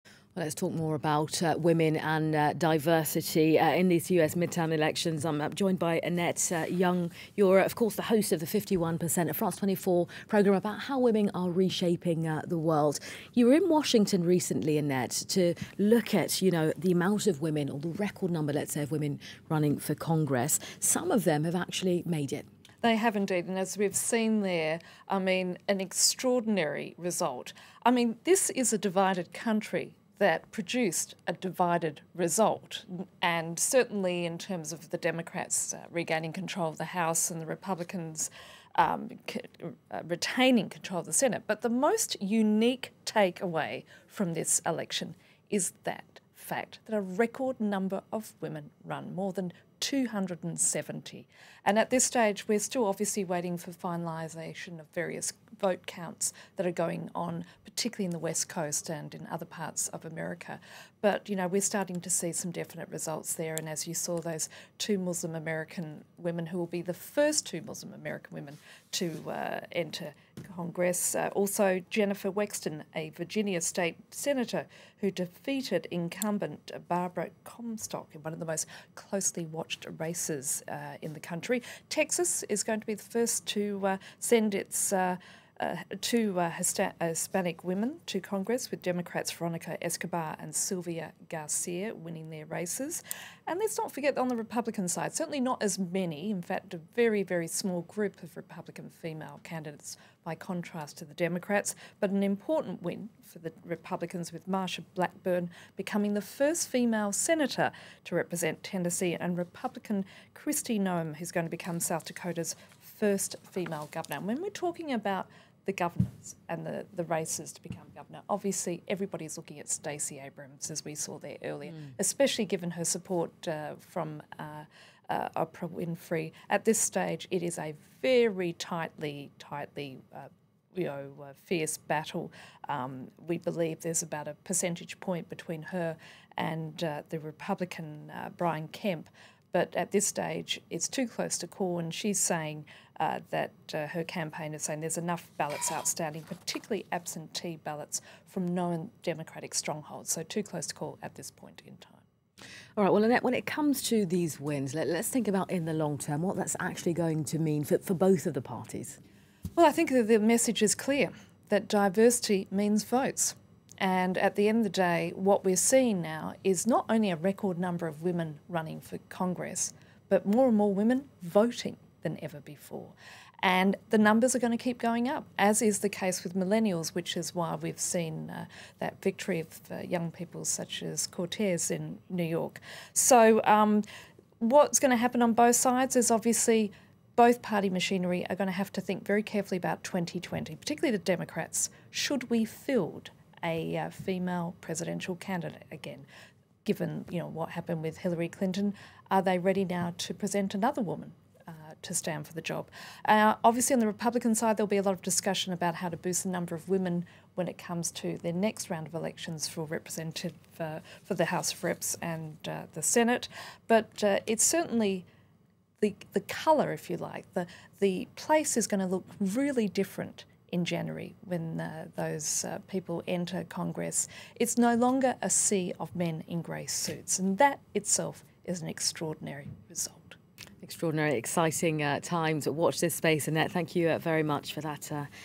US Midterms: "I think the message is clear, diversity means votes"
France 24's Annette Young talks about the record number of women that ran in this year's midterm elections.